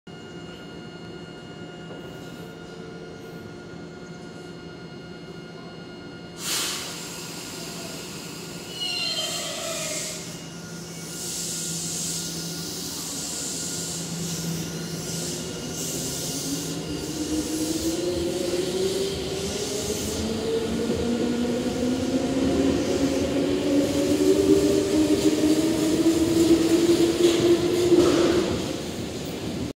㊗️ チャンネル登録者200人達成！！
200人ということで東武200系の動画をどうぞ。